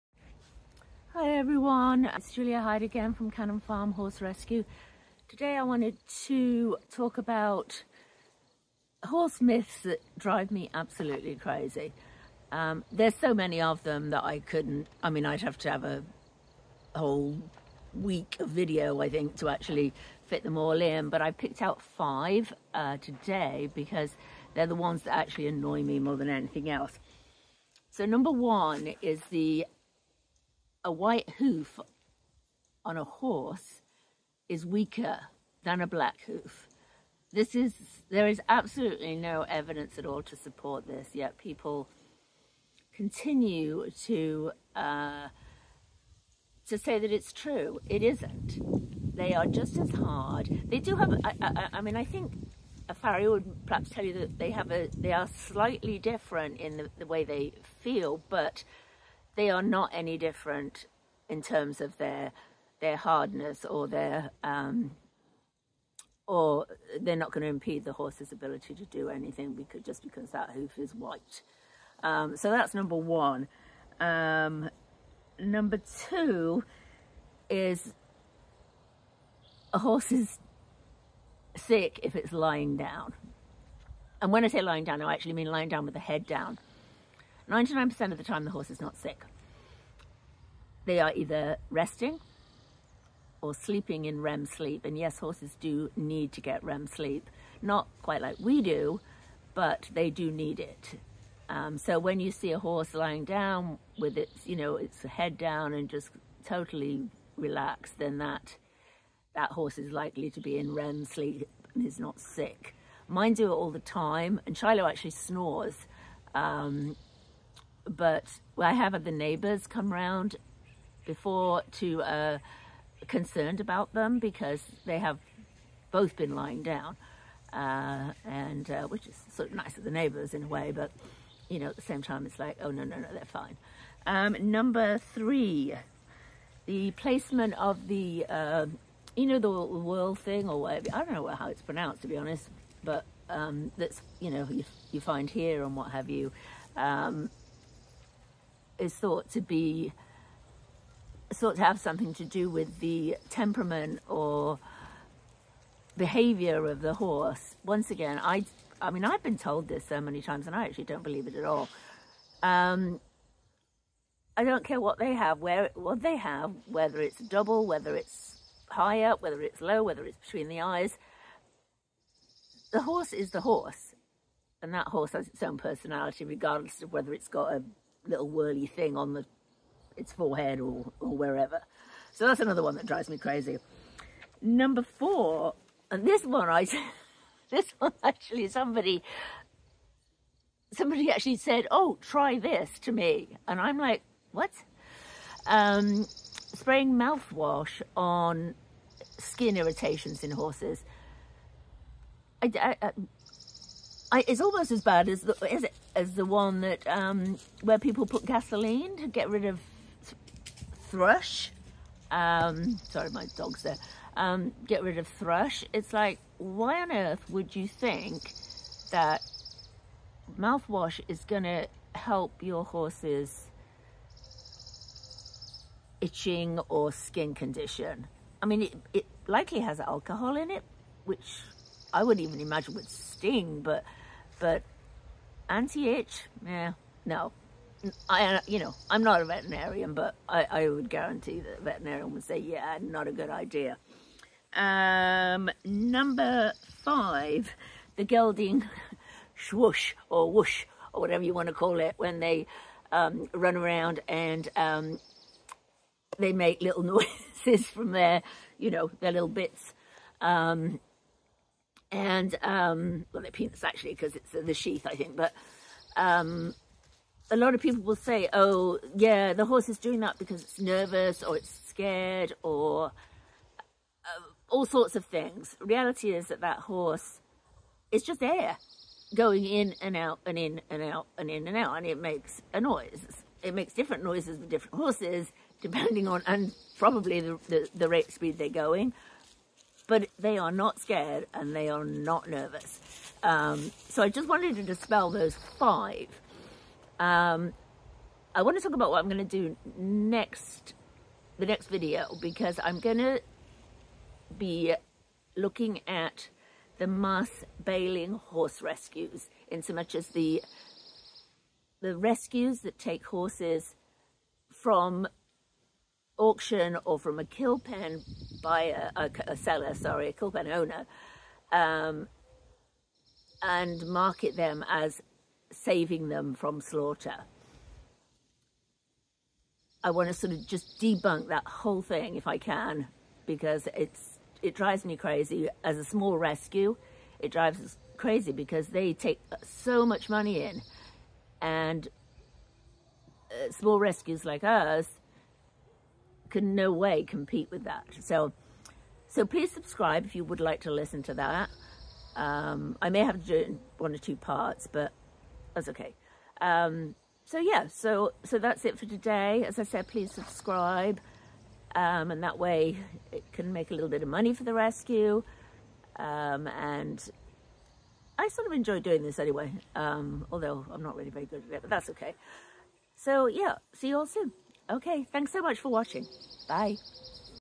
These 5 Horse Myths Drive Me Crazy (Do You Believe Any Of Them?)
There are so many horse myths circulating it's tough to just pick 5. I may have to make a part two.  And running a horse rescue and training horses brings some big ones to light. We have likely all heard of them but do you believe them or not?